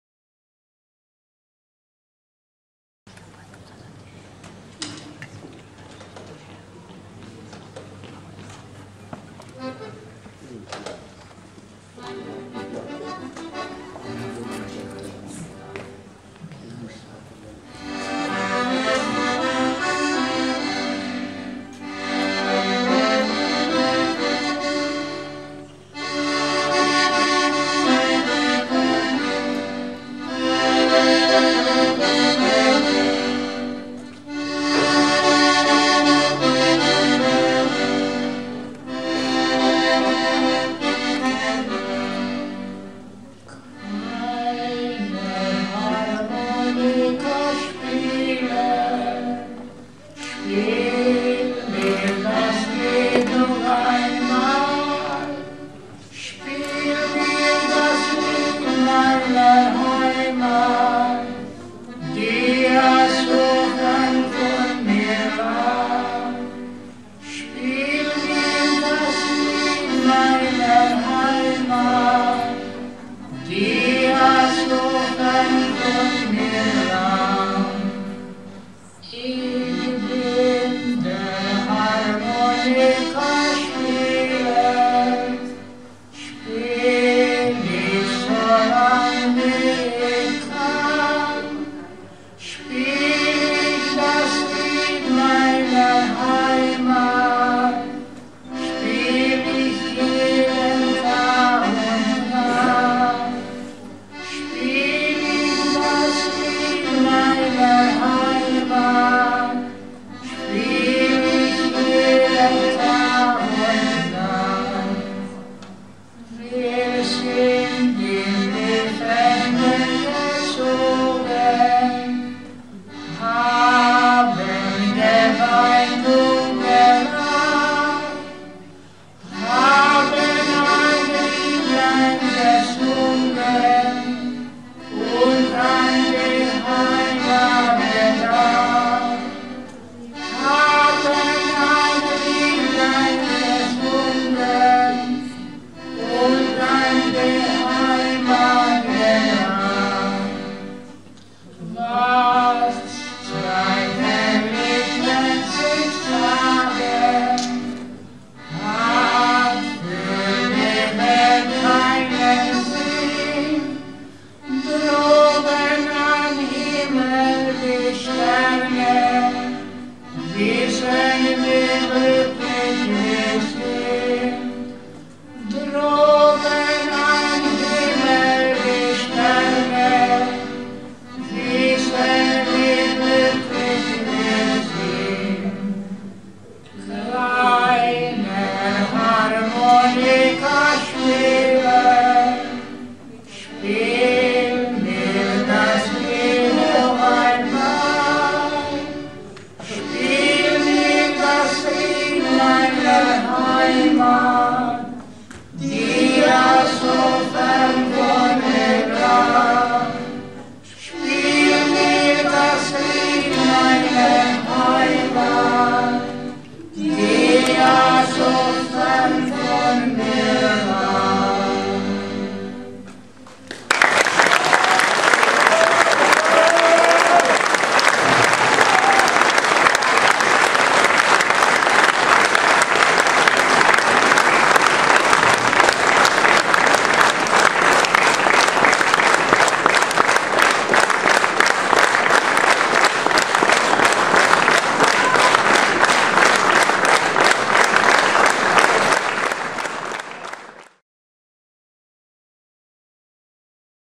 F. ROZSA und M. RITTLINGER beim TREFFEN DER MUSIKER MIT ALTEN INSTRUMENTEN in SOMOR 2009
AUS MEINEM ARCHIV: EIN SELTENES VIDEOZEITDOKUMENT
(noch unveröffentlicht)
 EIN WIEDERHÖREN/WIEDERSEHEN MITMUSIKLEGENDEN